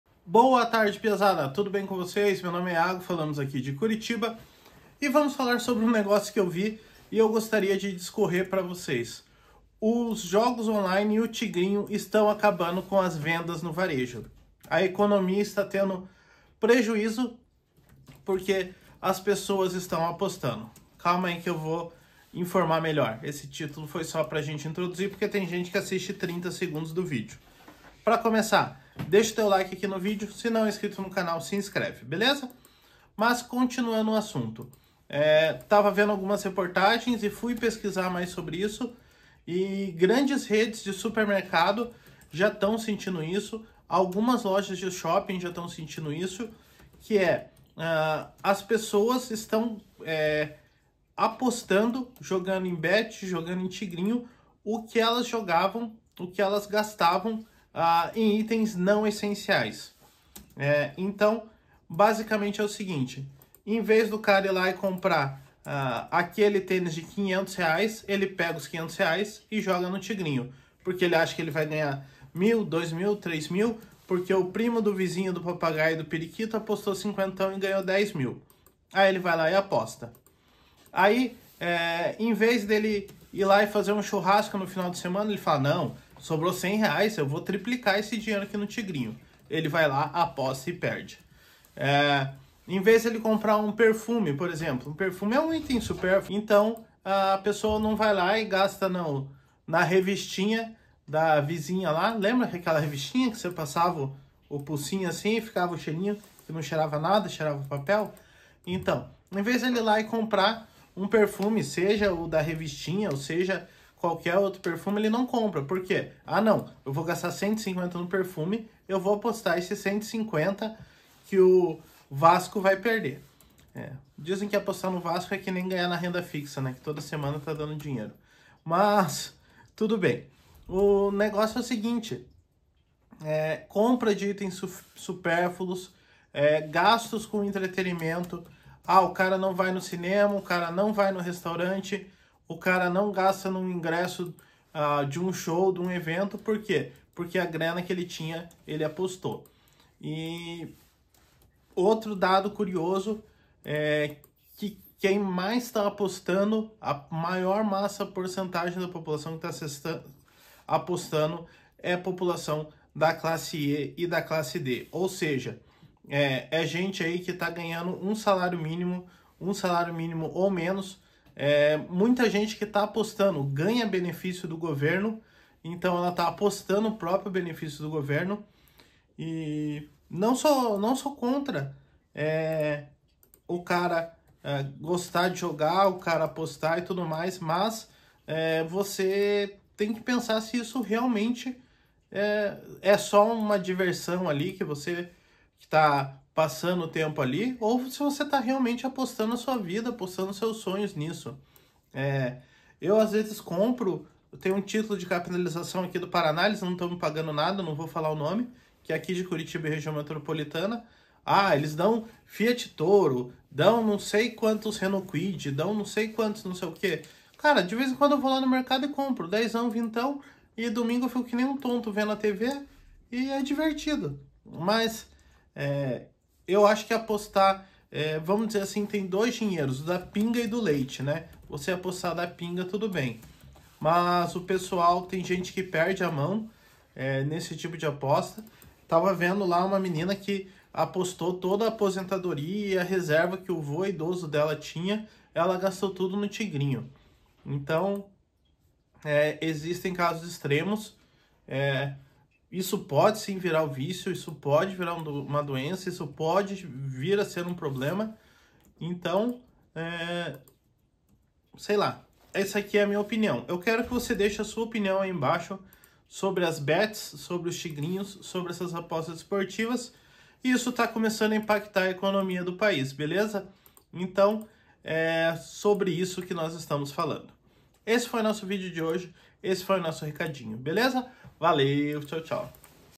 As Bets E O Tigrinho Estão Atrapalhando A Econômia
Os cassinos online e apostas, conhecidos como Tigrinho e Bets estão começando a impactar na econômia do País.